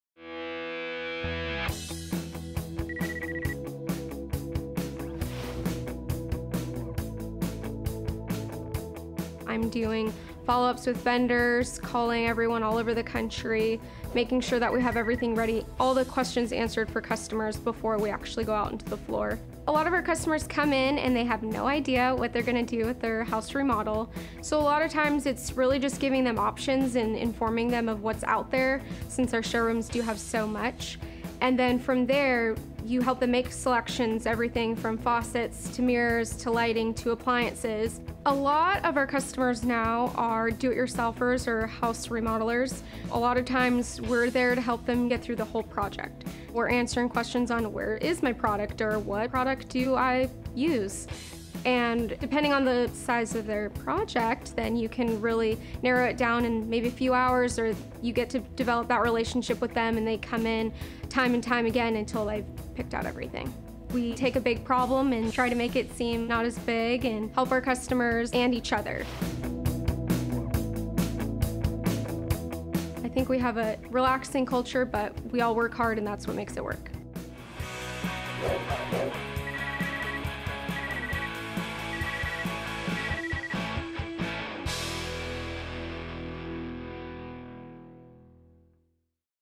Ferguson Trainee Program: Showroom Sales
The Sales Training Program is a 10-12 month program designed to educate, develop, and retain talented inside sales and showroom sales trainees. Once on-board, new associates start in a location where they complete a training program designed to develop knowledge, skills, and abilities as inside sales and showroom sales associates.

Upon completion of the Sales Training Program, sales associates will be responsible for managing all sales functions for existing and prospective customers. Typical job duties include: developing and maintaining customer relationships, preparing job quotes and providing accurate pricing/inventory information to customers.